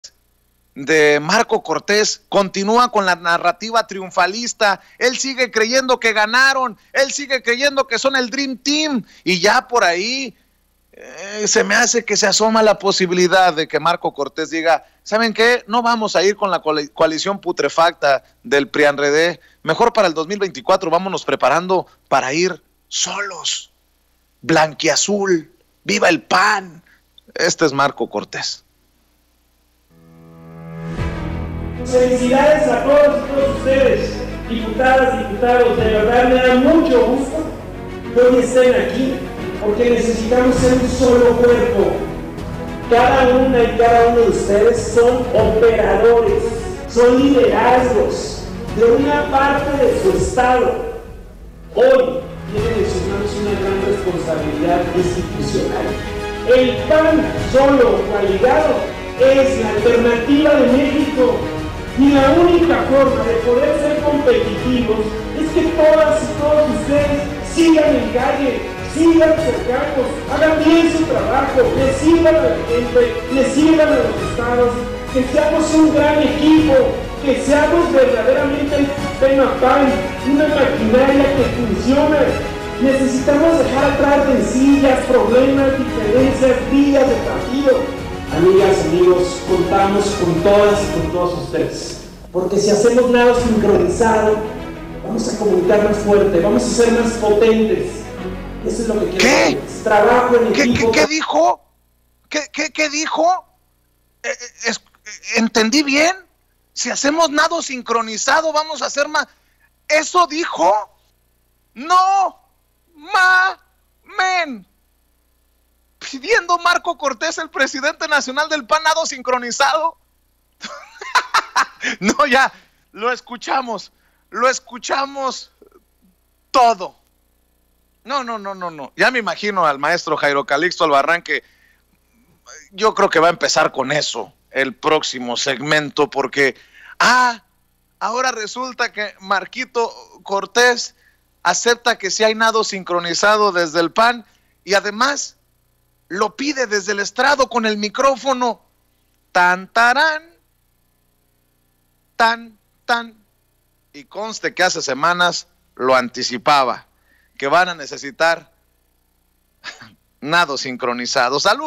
MECO CORTÉS ABIERTAMENTE PIDE NADO SINCRONIZADO… Y SIGUE CREYENDO QUE GANARON EN EL PAN
Marko “Meco” Cortés abiertamente pidió nado sincronizado a los militantes del PAN… Y sigue creyendo que ganaron. Chance y van solos en 2024 ¡Jajaja!